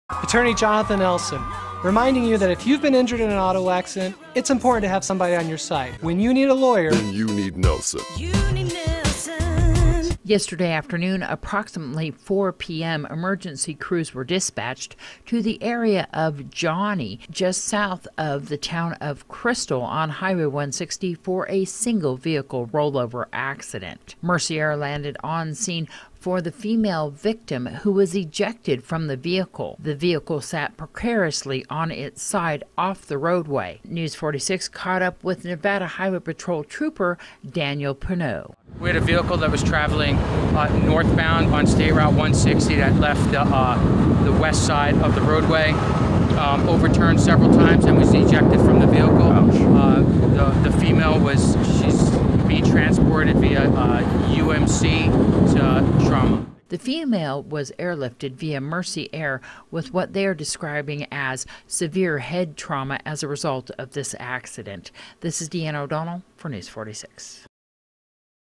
04/21/2016 Single Vehicle Accident Johnnie Curve
North Highway 160 was closed temporarily yesterday afternoon in order for emergency crews to work a single vehicle accident.